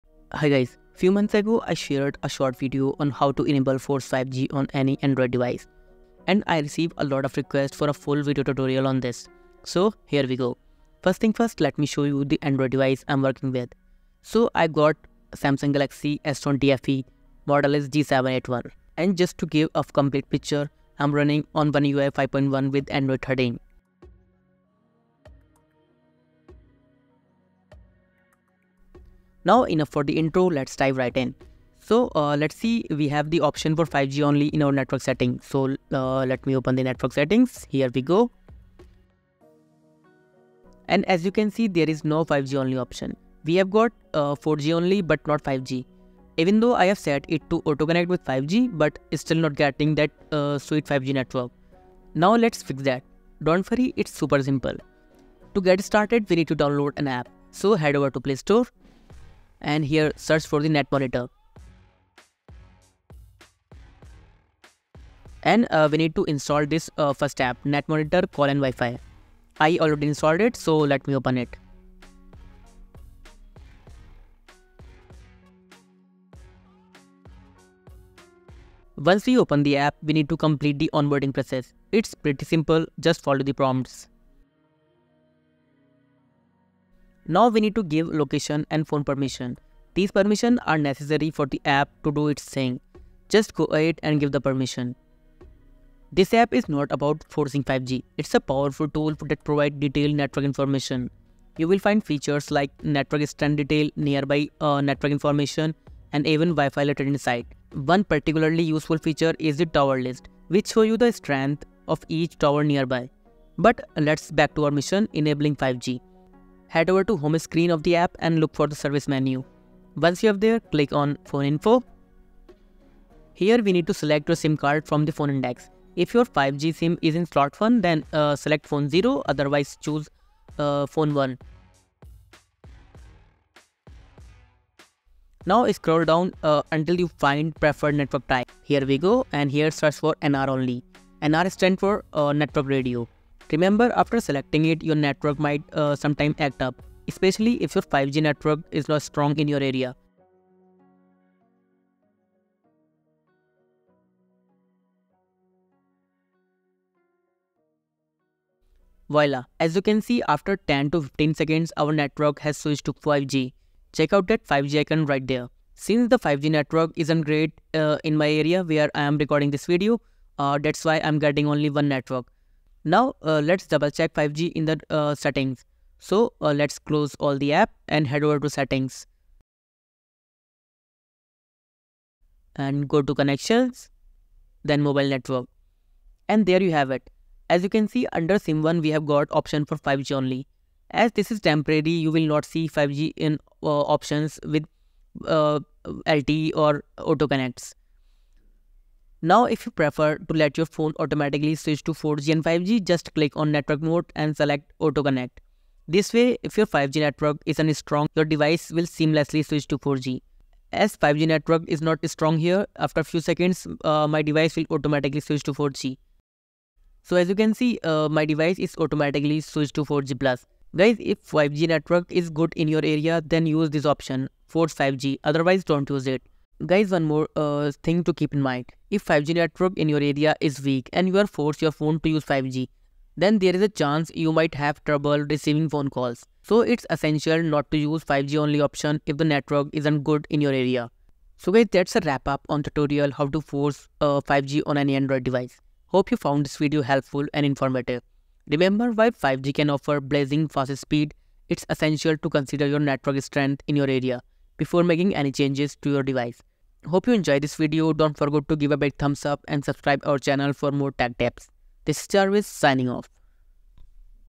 How to Force 5G on Any Android Device | 5G only samsung
force only 5G choose "NR" only
Force 5g Only mode (Android)
Only 5G network settings , Always on 5G network

Learn how to enable 5G on your Android device with this step-by-step tutorial! In this video, we'll show you how to force 5G connectivity on any Samsung device. Discover how to set your device to stay on the ultra-fast 5G network continuously by selecting "NR" only mode in your network settings.Follow our simple steps to ensure seamless connectivity to 5G.